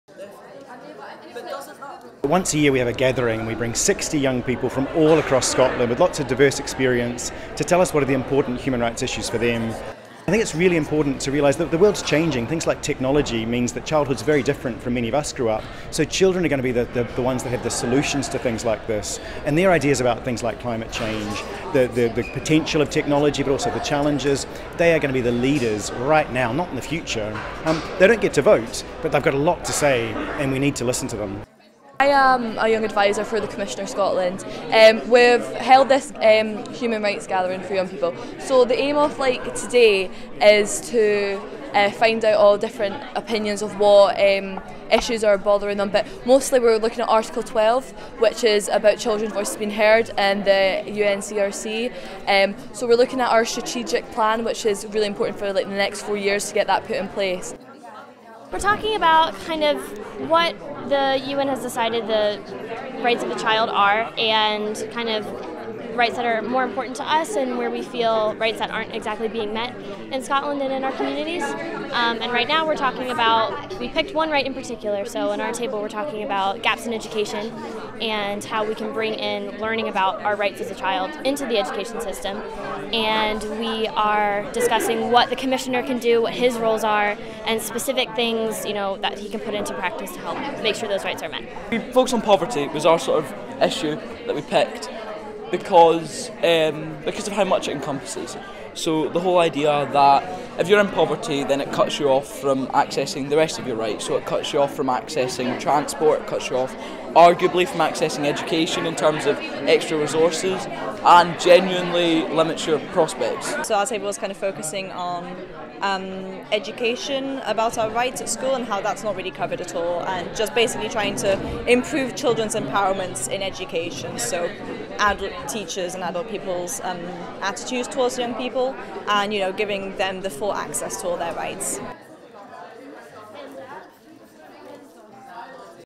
Young People's Human Rights Gathering 2019 (Unaired STV piece)
STV came to our Young People's Human Rights Gathering on November 16 to hear young human rights defenders explain why it matters for the young people in Scotland today. Unfortunately the piece never made it to broadcast, but STV have let us share their footage here.